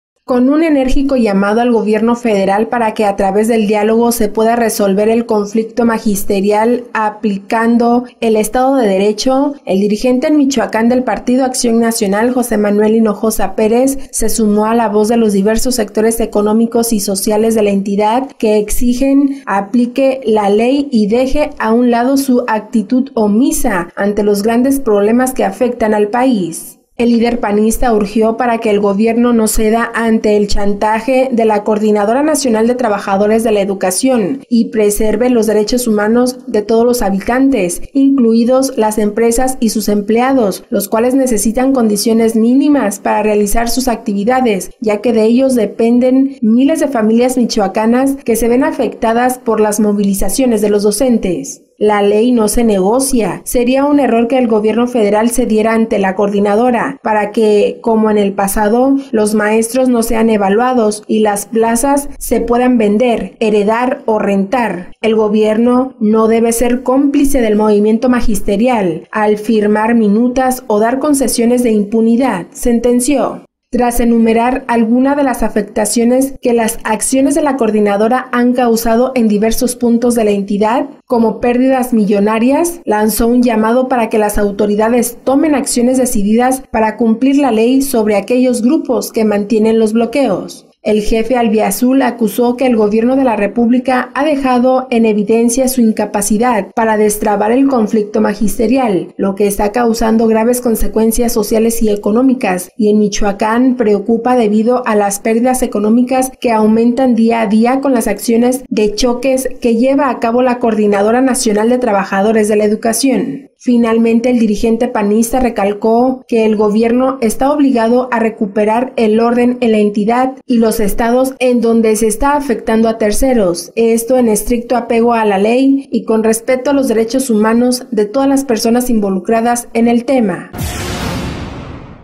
Insisten en garantías para el sector económico
-+El dirigente en Michoacán del Partido Acción Nacional (PAN), José Manuel Hinojosa Pérez, se sumó a la voz de los diversos sectores económicos y sociales de la entidad que exigen se aplique la ley y se deje de  lado la actitud omisa ante los grandes problemas que afectan al país.  Dijo que el gobierno federal debe buscar la solución con diálogo, pero aplicando el Estado de Derecho,